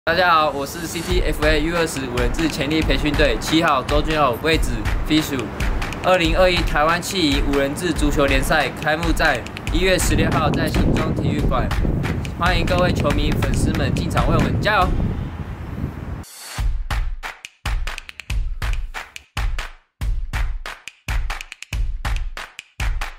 距離2021臺灣企銀五人制足球聯賽開幕戰還有8天！
CTFA U20潛力培訓隊的周峻佑邀請大家一起進場為他們加油！🤜🤛

2021臺灣企銀五人制足球聯賽開幕戰賽程

1月16日(六)
13:00 旅人FC VS CTFA U20
15:30 桃園國際 VS 芳億精密
18:00 曄拓FC VS 嘉義天晴

1月17日(日)
13:00 芳億精密 VS CTFA U20
15:30 曄拓FC VS 旅人FC
18:00 嘉義天晴 VS 桃園國際

地點：新莊體育館 (新北市新莊區中華路一段75號)

＊免費入場